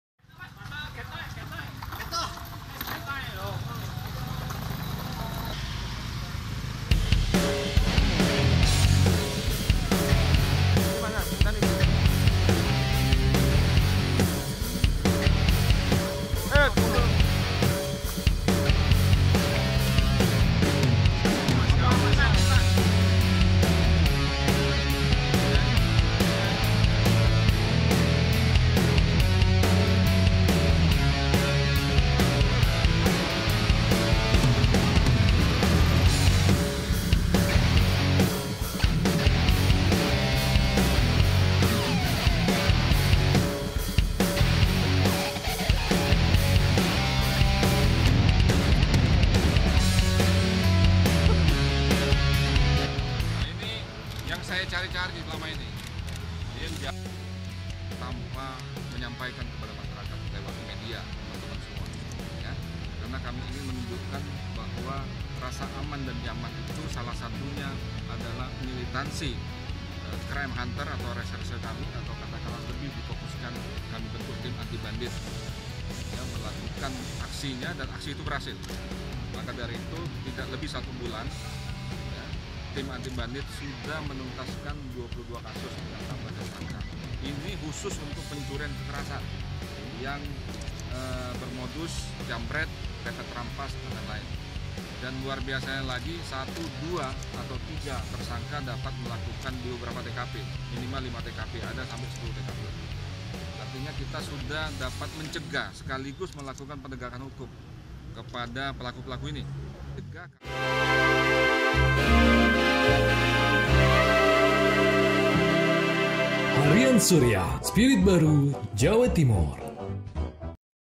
Polisi Surabaya Ciduk 16 Bandit, Ada yang Didooor
SURYA.co.id | SURABAYA - Genderang perang terhadap tindak kejahatan jalanan, terutama pencurian dengan kekerasan (Curas) dan jambret terus ditabuh Polretabes Surabaya.

Selama satu bulan mulai 27 Juni hingga 27 Juli 2017 tim Anti Bandit Polrestabes dan Polsek se-Surabaya mengungkap 22 kasus dengan 16 tersangka.

Tersangka sebanyak itu, rincinanya Curas sebanyak 2 kasus dengan 8 tersangka. Sedangkan jambret ada 14 kasus dengan jumlah tersangka sebanyak 8 orang. Ke-16 tersangka yang kini menghuni sel tahanan di Polretabes dan Polsek itu, usianya mulai 18 tahun hingga 35 tahun.

"Kami menyampaikan rasa aman dan nyaman kepada masyarakat. Bentuknya militansi dari tim Anti Bandit yang terus beraksi menindak tegas penjahat dan berhasil.

Dalam waktu kurang satu bulan bisa mengungkap 22 kasus Curas dengan 16 tersangka. Sebagian besar pelaku jambret dnegan modus pepet dan rampas," sebut Kapolretabes Surabaya, Kombes Pol Mohammad Iqbal, Kamis (27/7/2017).

Menurut Iqbal, para pelaku tidak memilih tempat untuk beraksi. Semua tempat di Surabaya patut diwaspadai lantaran pelaku bsia saja beraksi jika kesempatan berbuat jahat memungkinkan.

Dari 16 pelaku yang di ringkus, mereka telah beraksi di Jl Tidar, Jl Bubutan, JL Tembok Dukuh, JL Kalikepiting, JL Raya Tanjungsari Sukomanunggal, Jl Babat Jarawat Pakal, Jl Indrapura, JL Rajawali, JL pahlawan, Jl Pasar Kembang, JL Arjuno, JL Karangmenjangan, Jl Wonokromo, Jl Ngaglik, dan Jl Sumatera.

"Ada pelaku yang melakukan kejahatan di lima sampai 10 TKP dan berhasil ditindak. Kami
sudah bisa mencegah sekaligus melakukan penegakan hukum kepada pelaku-pelaku ini," tegas Iqbal.

Mantan Kpolres Gresik dan Sidoarjo ini memberi peringatan kepada penjahat supaya tidak main-madin di Surabaya. Pihaknya akan melakuakn tindakan tegas para pelaku kejahatan jalanan.

"Kami akan sikat dengan tegas. Tim Anti Bandit berjumlah 70 orang dibantu 23 Polsek jajaran, sepanjang hari mengintai peklaku. Masyarakat harus bisa merasa amanan," ucap Iqbal.

Selain meringkus 16 tersangka, tim Anti Bandit berhasil menyita barang bukti berupa hasil curian dan lainya. Seperti 10 motor dari berbagai merek dan jenis, jam tangan, 5 hanphone (HP), tas, dompet dan barang bukti lainnya. FATKUL ALAMY